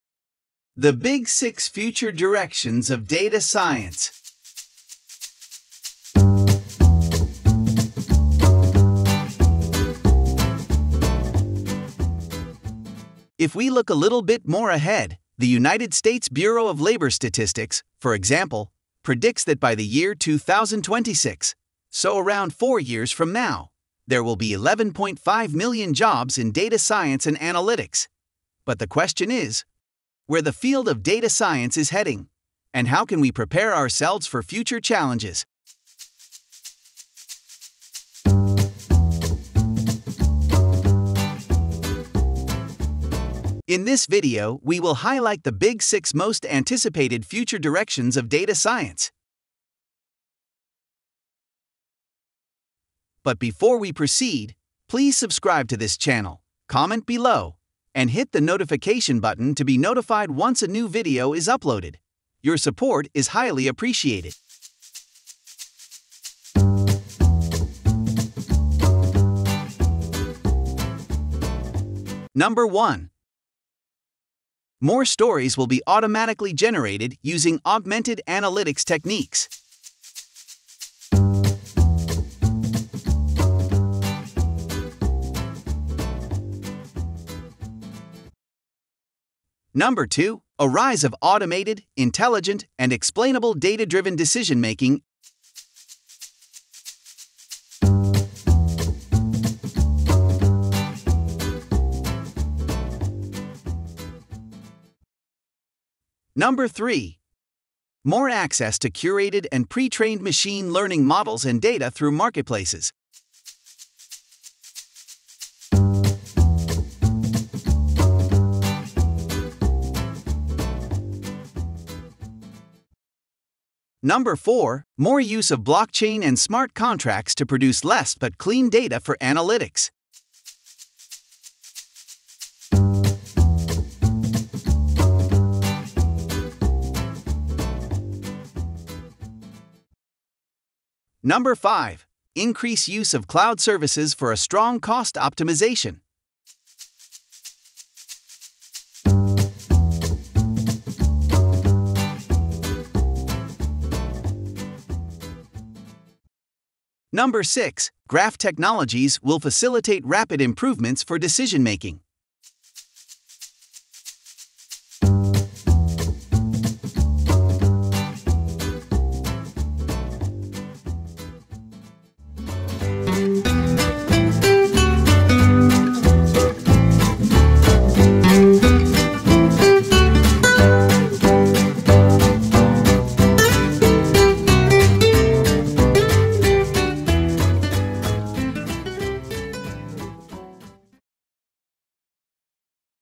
The Big 6 Directions of Data Science
In this video, we highlighted the big 6 most anticipated future directions of data science. More details will be introduced in future videos. Your support is highly appreciated.